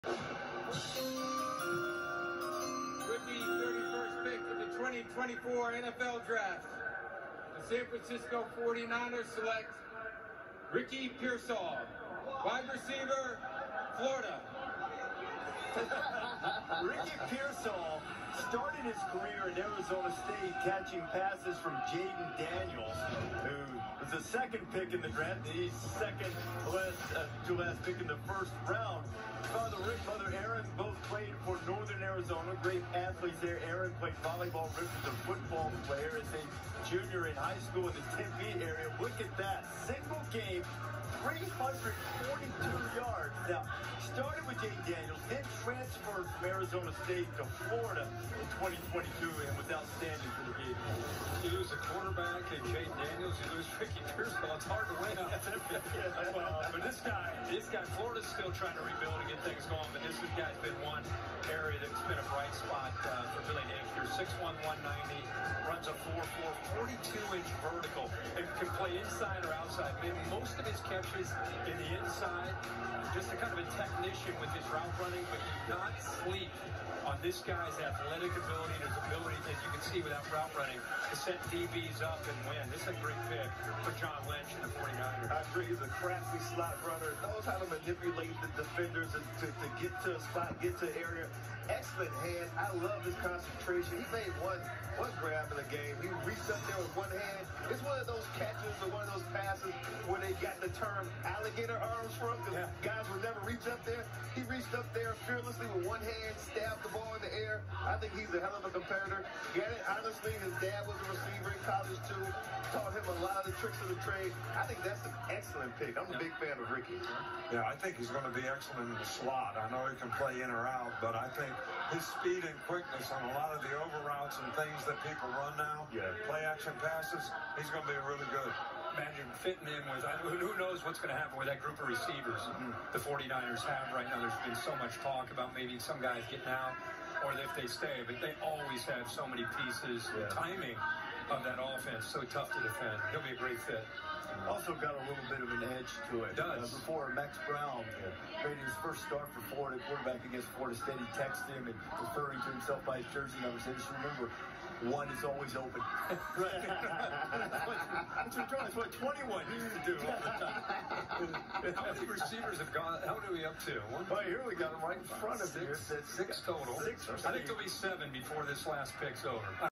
Niner 9er fan straight up boos, does not like first round pick Pearsall (long version)
Give the young man a chance. Go 9ers!